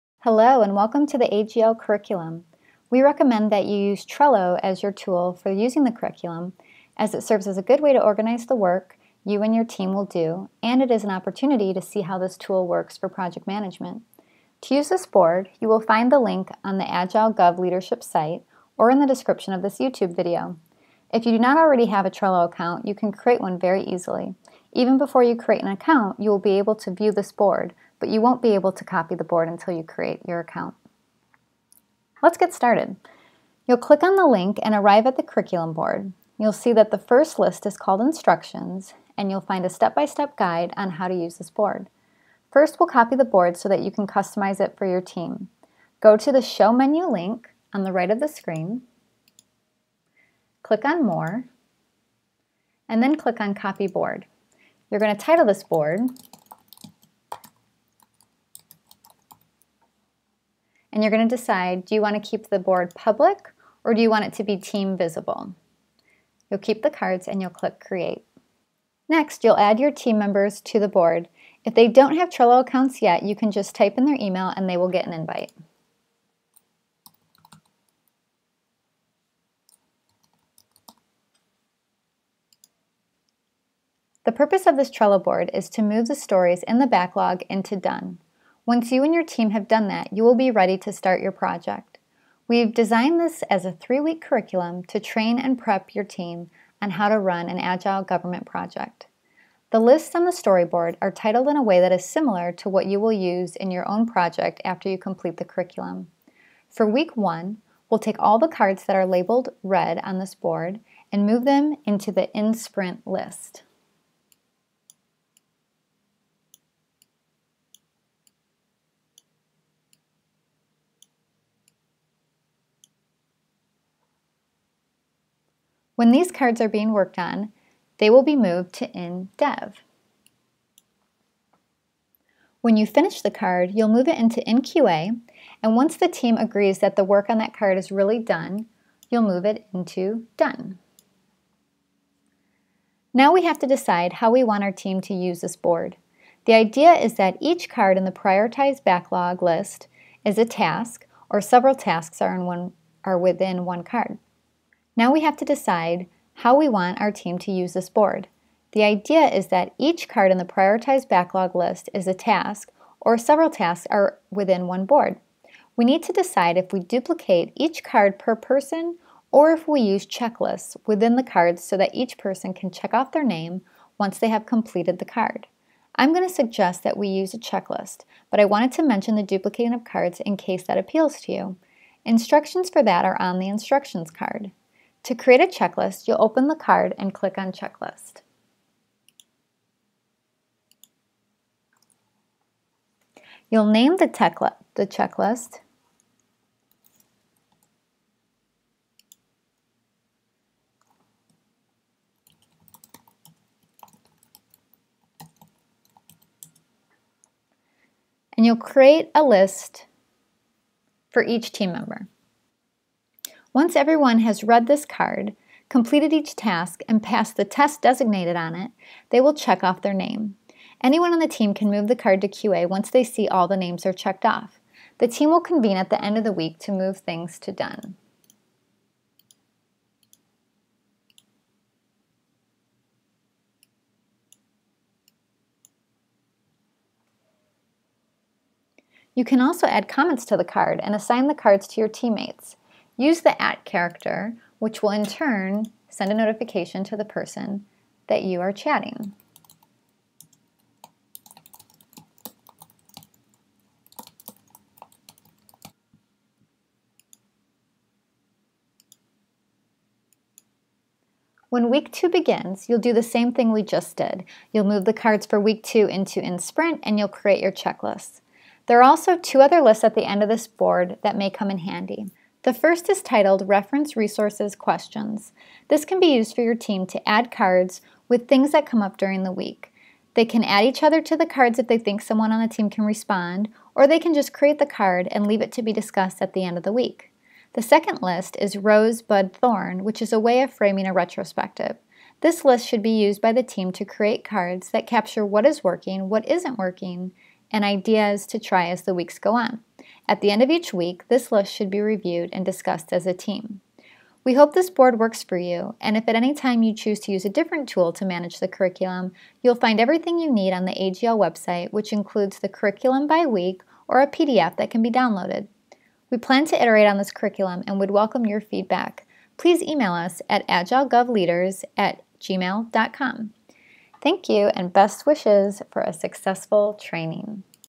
Trello for AGL Academy
How to use Trello for the AGL Academy course 'Agile for the Government Project Manager'.

Created 1/29/2016.